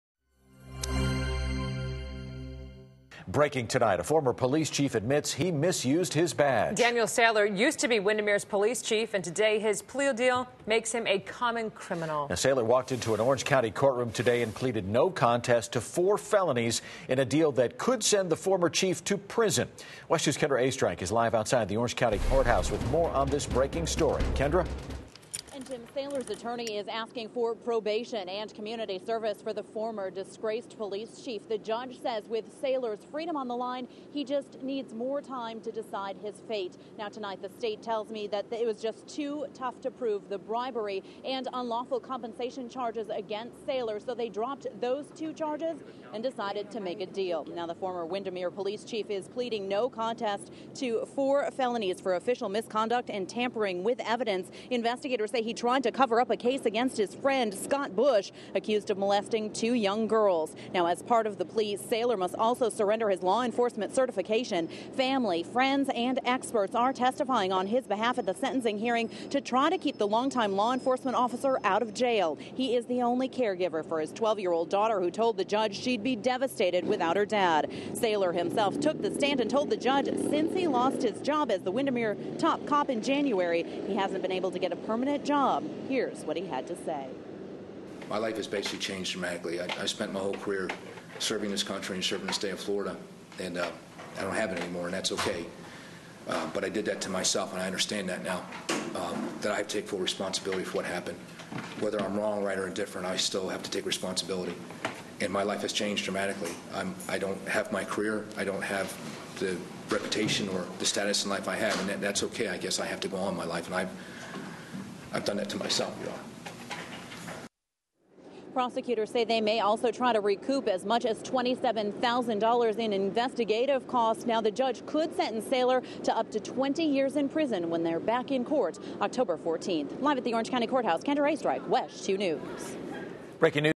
Axed Windermere Police Chief Reaches Plea Deal
Former Windermere Police Chief Daniel Saylor has reached a plea deal following charges of bribery, misconduct and tampering.